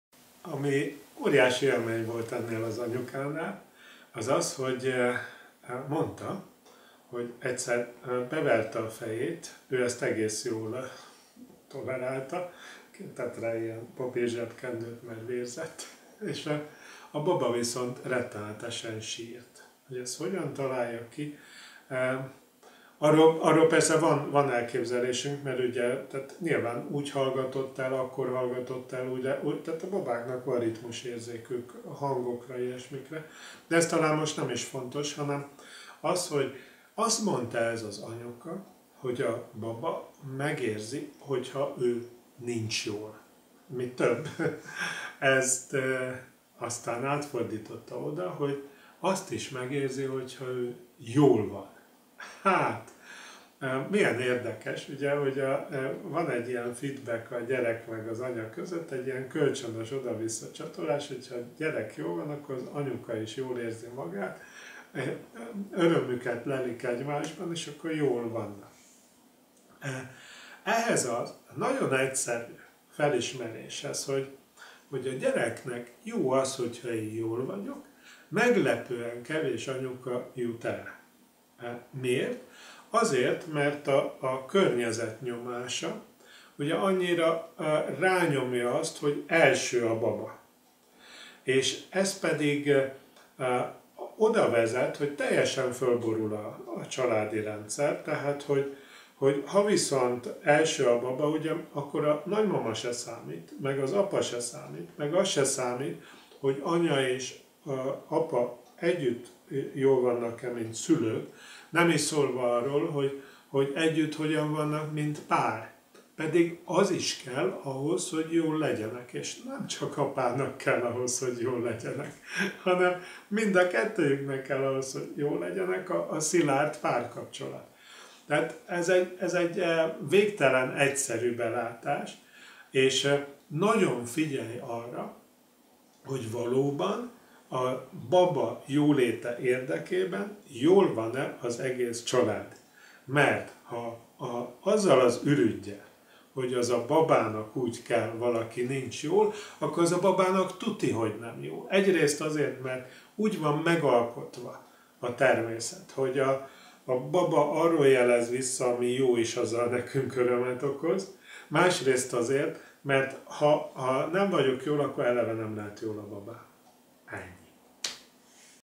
Önerősítő körök a család hogylétében
Miért sír a baba, amikor az anyuka üti be a fejét? Miért fontos, hogy jól legyen az anyuka, sőt, jól legyen az egész család?

Ha szeretnél bekapcsolódni a Szülők kézikönyve programba, várunk a honlapon vagy facebook csoportban.

Ha személyesen szeretnéd velünk továbbgondolni/beszélni a témát, megtalálod hozzánk az utat a oldalon.